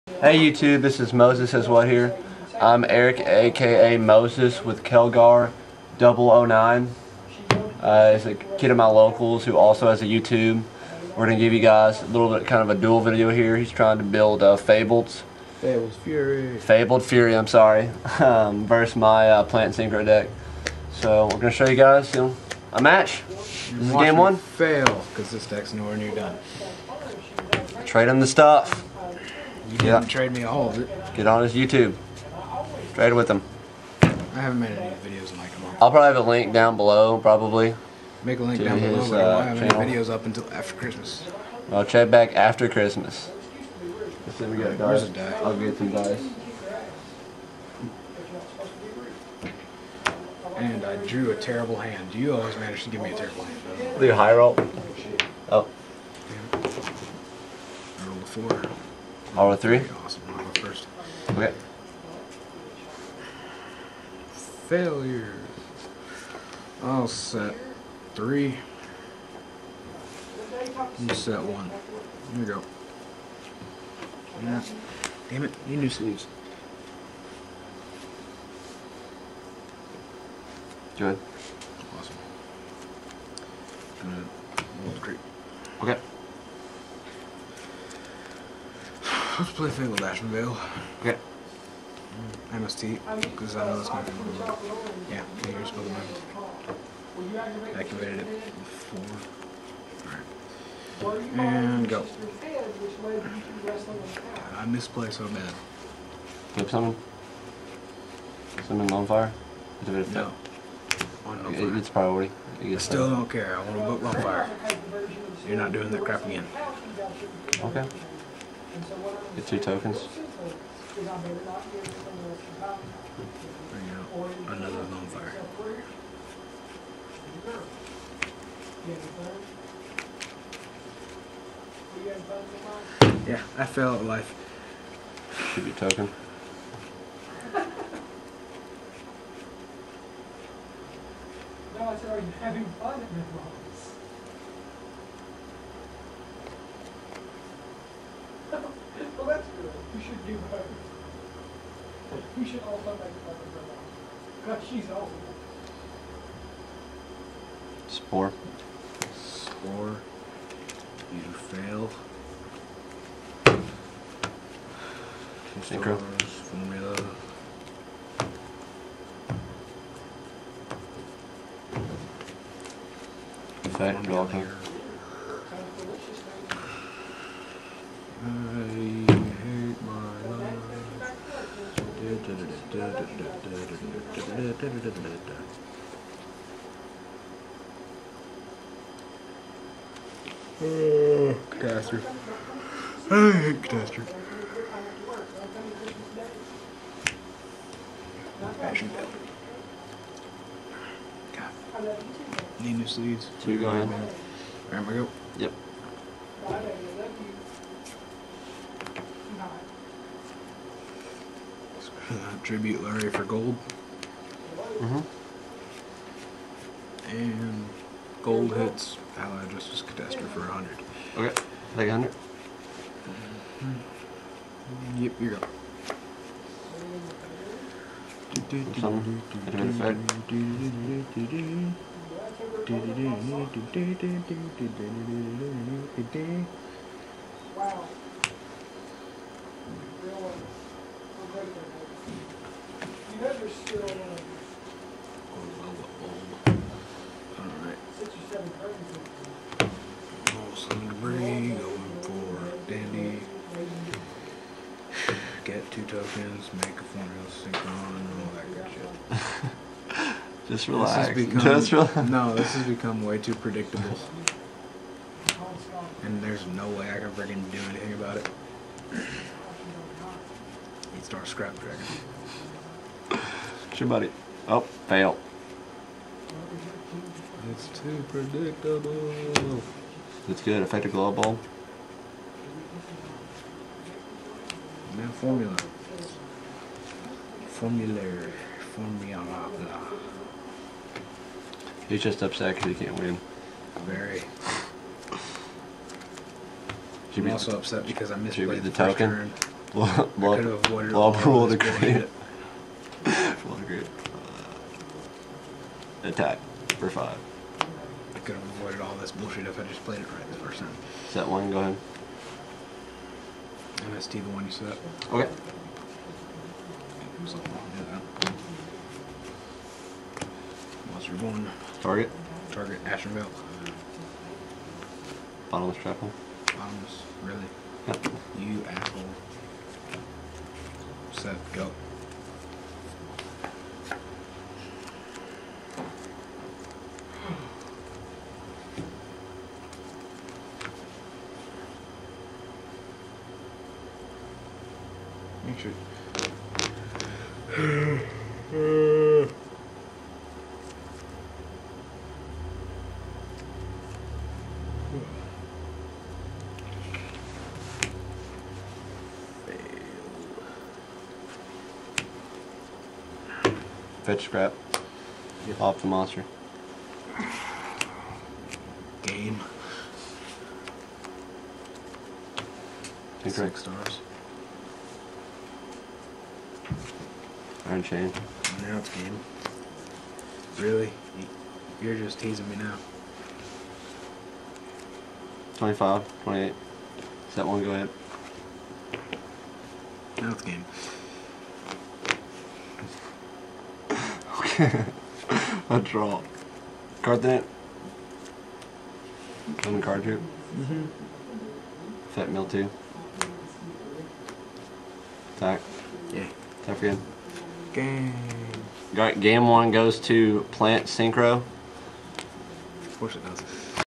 Aeric Pence (Plant Synchro) vs Dustin Wheat (Fabled Fury) Game 1 @ Heroes Realm
Hey YouTube! This is MosesSaysWhat here bringing you all a match between Fabled and Plant Synchro at Heroes Realm. The match itself was more about fun than anything, but as you can see the game gets fairly competitive. Big thanks to Dustin (Kelgar009) for letting me get a match in with him. Lastly, I apologize for the glare on Dustin's graveyard. We didn't notice that until we were done taping :( With all that being said...Enjoy!!